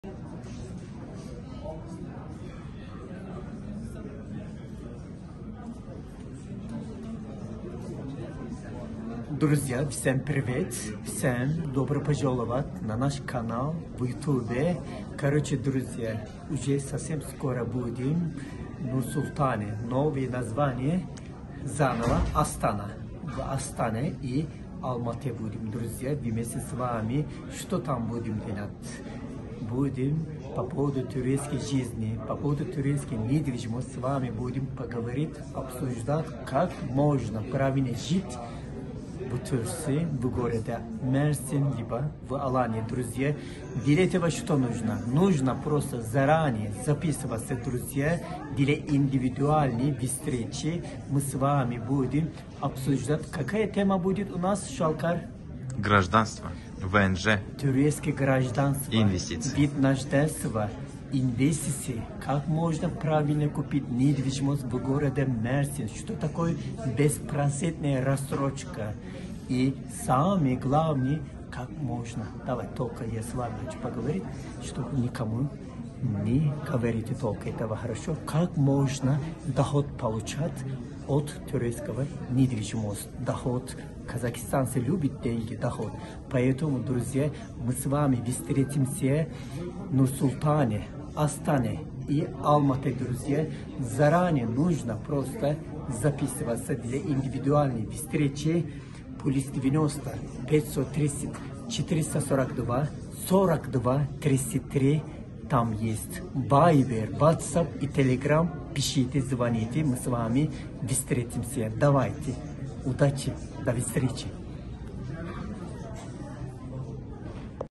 🇰🇿 МЫ В КАЗАХСТАНЕ / Недвижимость в Турции / Аланья и Мерсин
💡 Что такое апостиль? Как правильно его оформить?

▶️ Какой район в Мерсине выбрать для жизни?

▶️ Как не попасть на мошенников и не потерять свои деньги при покупке недвижимости?

▶️ Как в Турции передается недвижимость по наследству?

▶️ Чем можно заниматься в Мерсине?